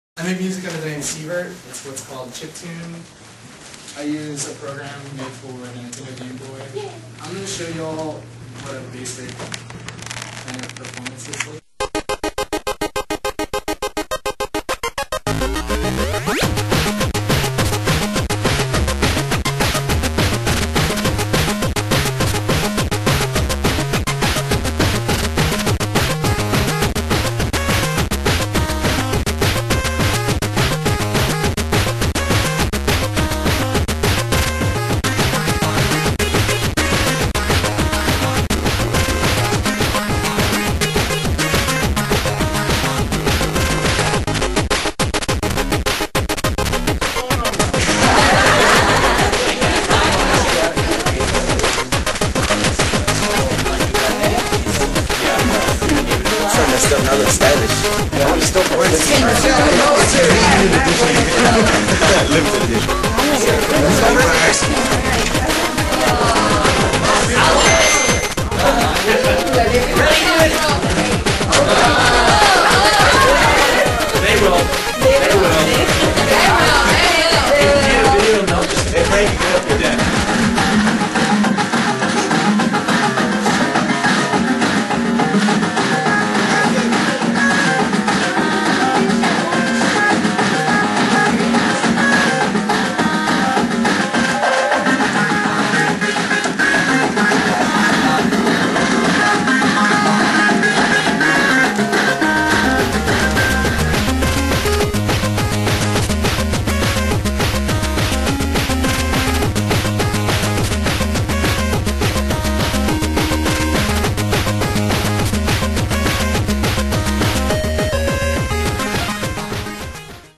Chiptunes
Aldine Branch Library hosted a special program in conjunction with Teen Read Week. This year's theme was "Books with Beat" and a local artist visited the library to demonstrate how old video game consoles can be rewired to be used to make music!